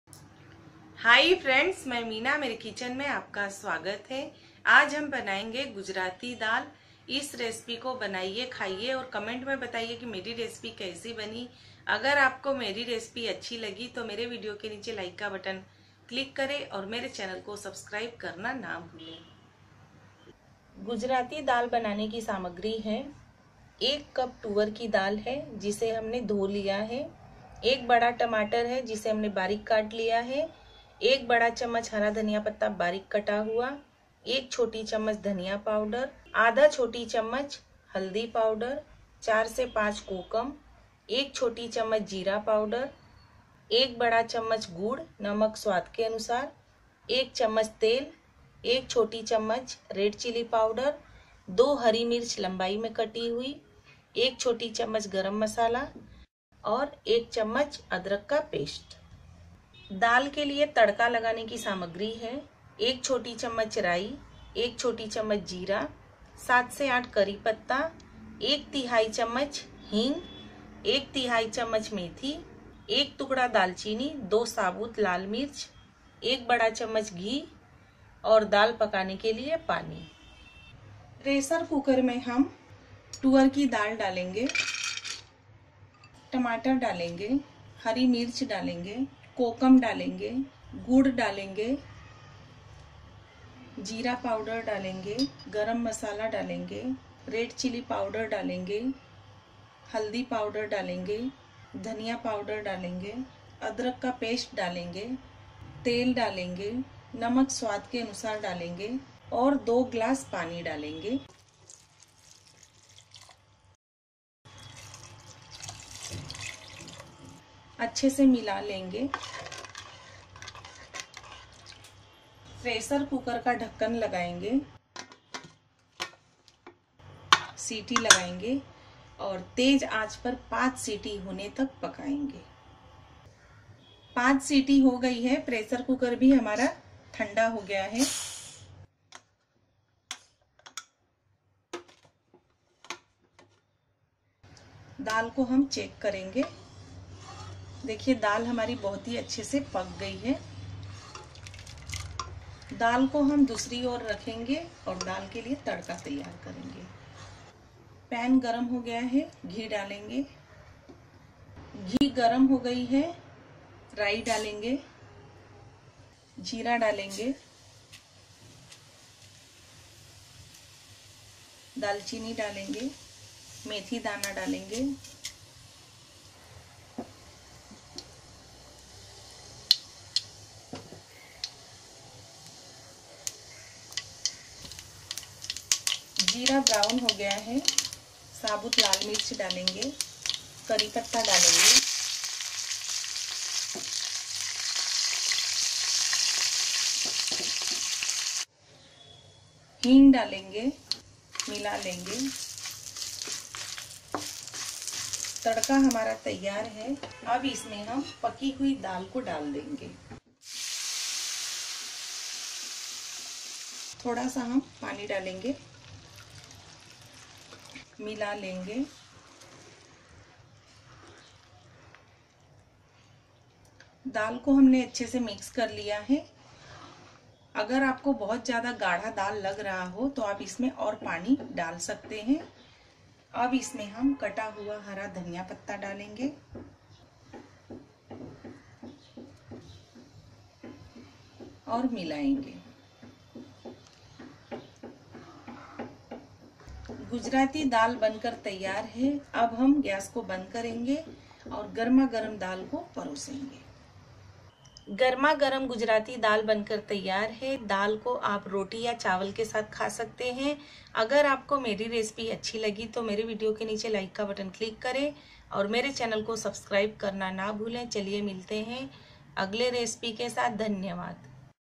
Gujarati Dal Recipe - गुजराती दाल - Gujarati Toovar Dal
Ingredients :-
For Pressure Cooking :-
Toor Dal - 1 cup
Finely chopped Tomato - 1
Finely chopped Coriander leaves - 1 tbsp

For Tempering :-
Meethi seeds - 1/3 tsp
Water

सामग्री :-
प्रेशर कुकिंग के लिए: -
तूर दाल - 1 कप
बारीक कटा हुआ टमाटर - 1
बारीक कटा हरा धनिया - 1 बड़ा चम्मच
कोकम - 4 से 5
जीरा - 1 चम्मच
गुड़ - 1 बड़ा चम्मच
नमक स्वादअनुसार
कटी हुई हरी मिर्च - २
गरम मसाला - 1 चम्मच
अदरक का पेस्ट - 1 बड़ा चम्मच

तड़के के लिए :-
सरसों - 1 चम्मच
जीरा - 1 चम्मच
करी पत्ते - 7 से 8
हींग - 1/3 चम्मच
मेथी के बीज - 1/3 चम्मच
दालचीनी - 2 टुकड़े
सूखी लाल मिर्च - 2
घी - 1 बड़ा चम्मच
पानी